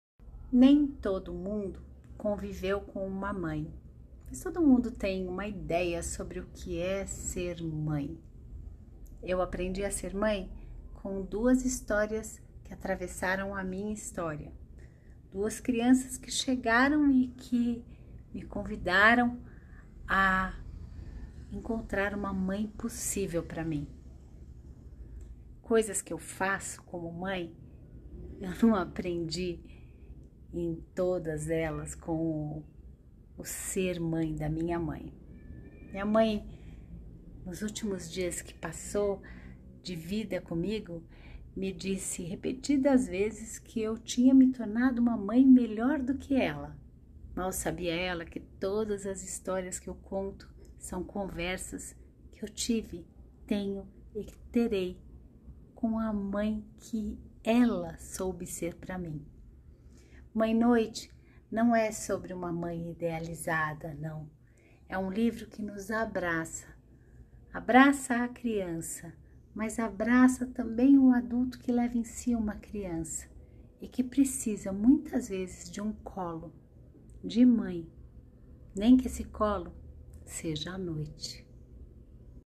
INSPIRAÇÕES E VIVÊNCIAS DE PENÉLOPE MARTINS, AUTORA DE "MÃE NOITE"
Passado o fim de semana de Dia das Mães, confira o relato de Penélope Martins, autora de "Mãe noite", sobre suas inspirações e vivências que a motivaram a escrever o livro.